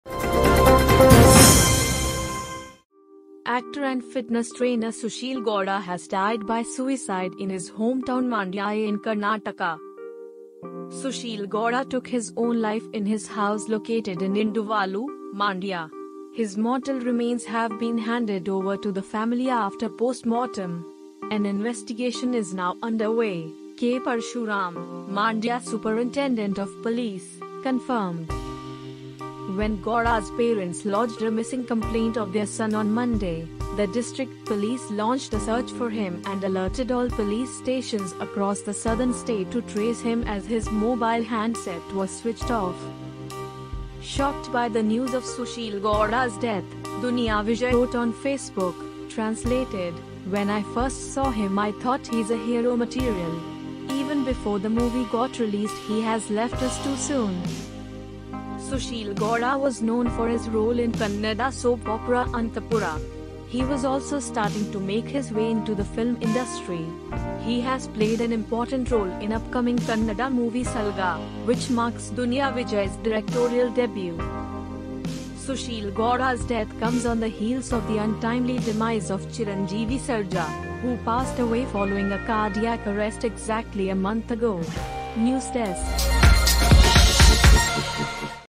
Popular Kannada actor Susheel Gowda commits suicide, fans and colleagues shocked
Actor and fitness trainer Susheel Gowda has died by suicide in his hometown Mandya in Karnataka.The exact reason for suicide hasn't been revealed yet. The news has come as a shock to many celebrities. The actor was 32 years of age and had acted in TV shows.